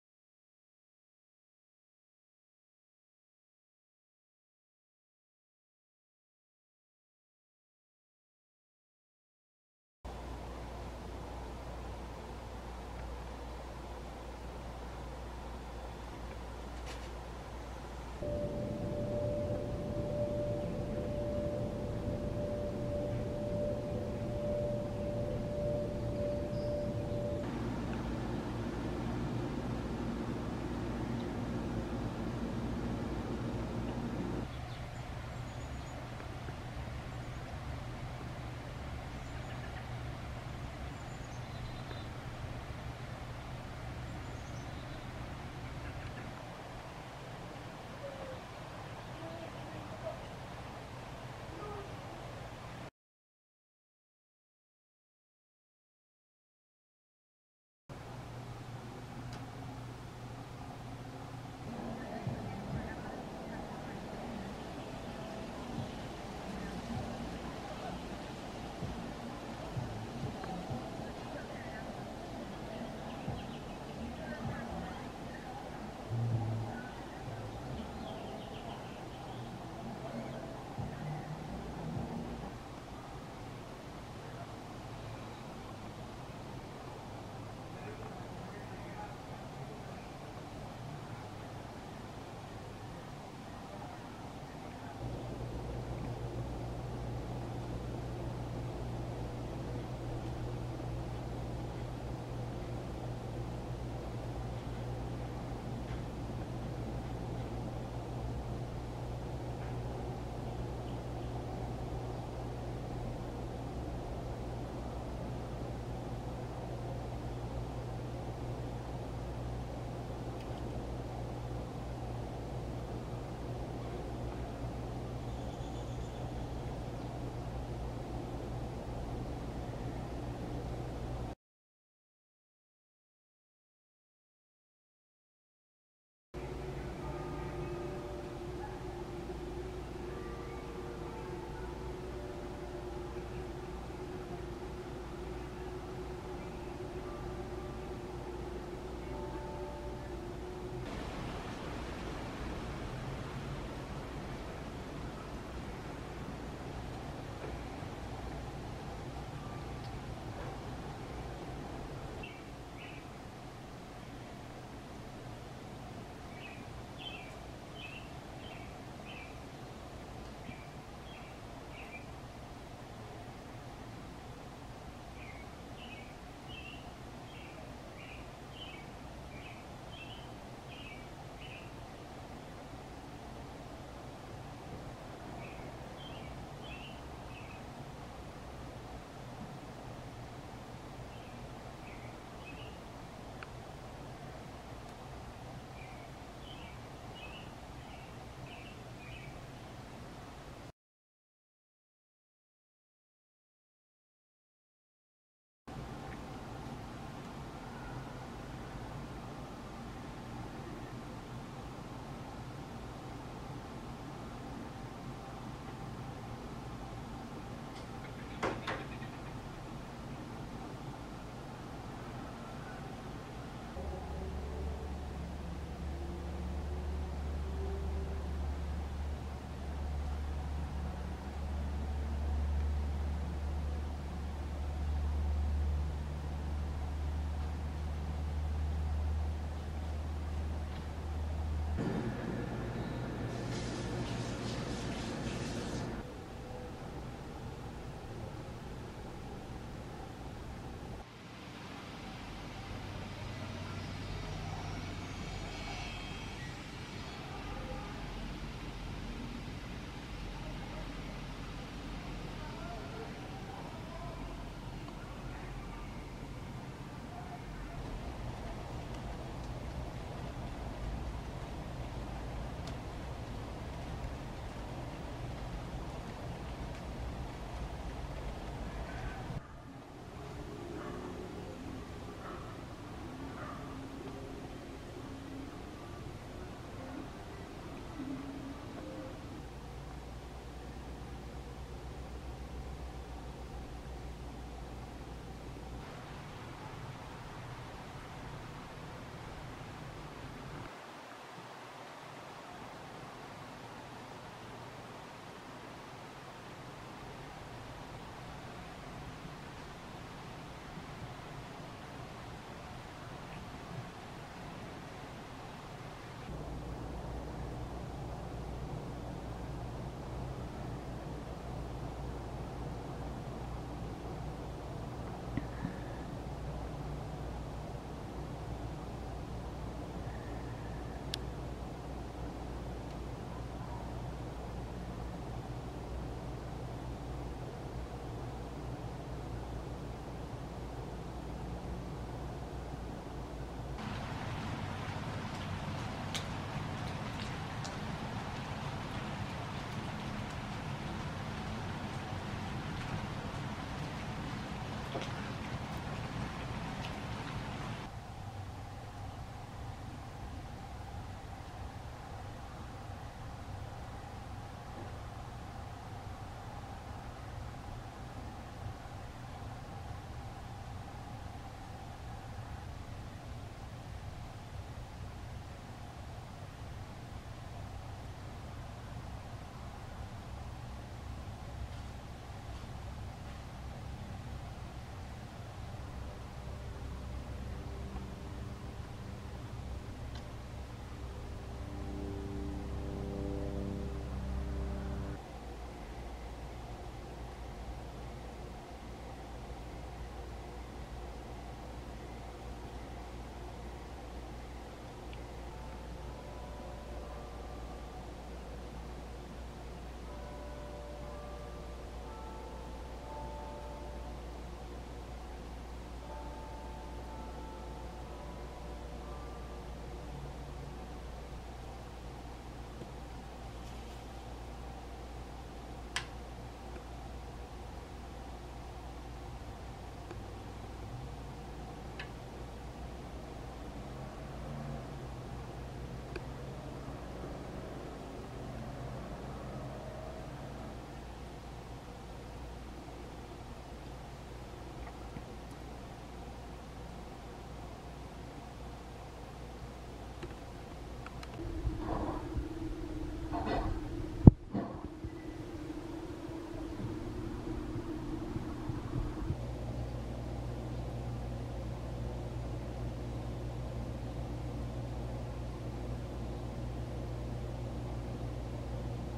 Squirrels in my backyard, Salt Lake City - Part 2
Activities in this video: Nibbling, Eating buds, Eating maple blooms, and Eating maple seeds

Video footage by Chris Fountain and Janet Fountain
Artwork by Janet Fountain